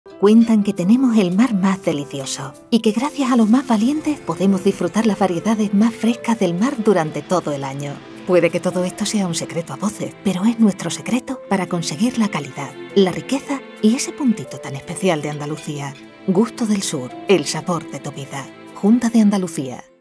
Productos Pesqueros. El sabor de nuestros mares y océanos [Gusto del Sur, 5/11]
Surcan nuestros mares, océanos y costas. Es disfrutar hasta las espinas. Ese es nuestro secreto a voces. Tierras bañadas por el Mediterráneo y el Atlántico, surtiéndonos de delicias de mar aclamadas mundialmente.